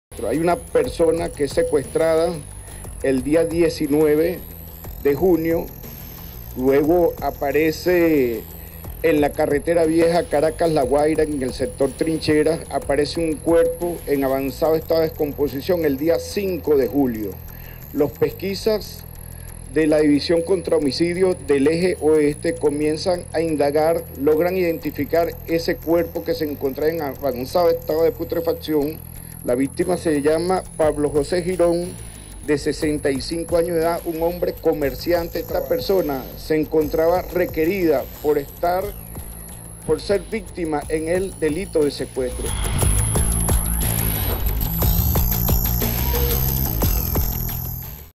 Balance semanal (Cicpc): Detenidas 4 personas por caso de secuestro
Un total de 4 personas resultaron detenidas por el secuestro y homicidio del ciudadano Pablo José Girón de 65 años de edad.